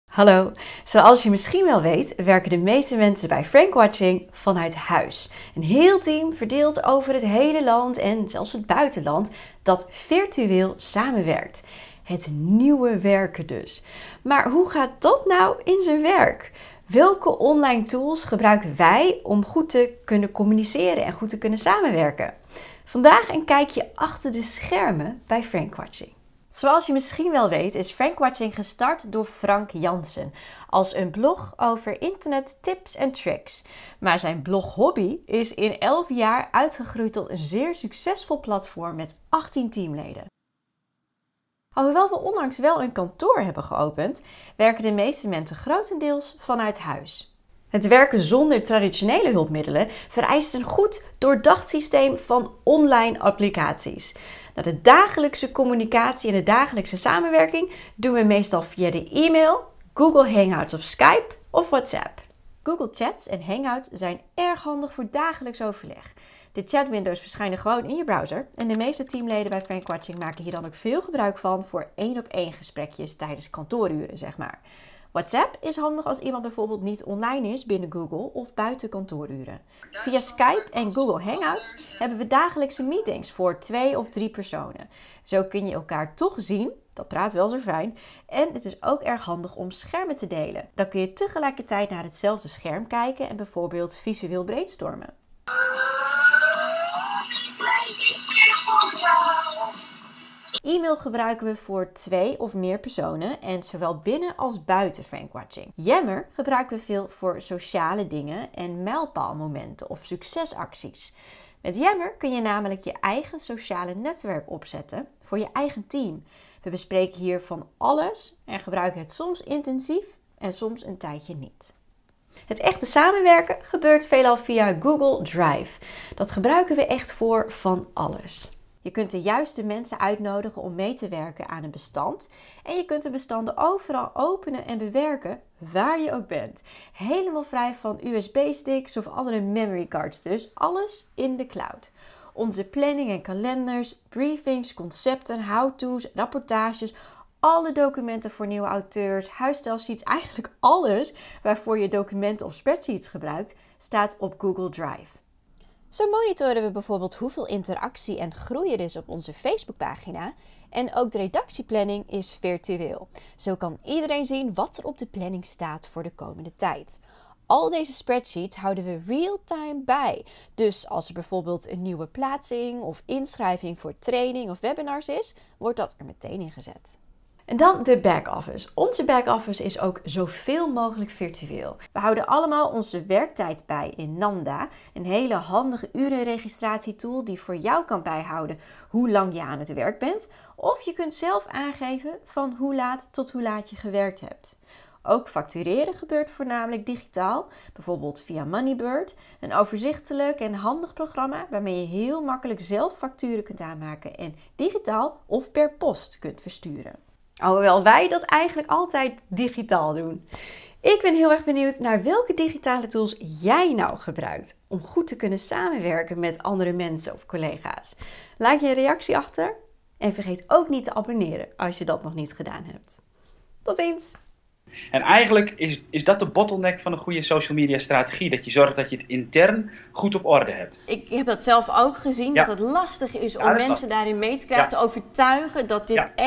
Frankwatching achter de schermen: samenwerken met handige online tools
Werken vanuit huis, daar kun je wel wat handige online tools bij gebruiken. Zeker als je dat doet met 18 collega's. In deze video's een kijkje achter de schermen van Frankwatching: tips & tricks voor online samenwerken. Leuk dat je keek! Vergeet niet op 'like' te klikken :)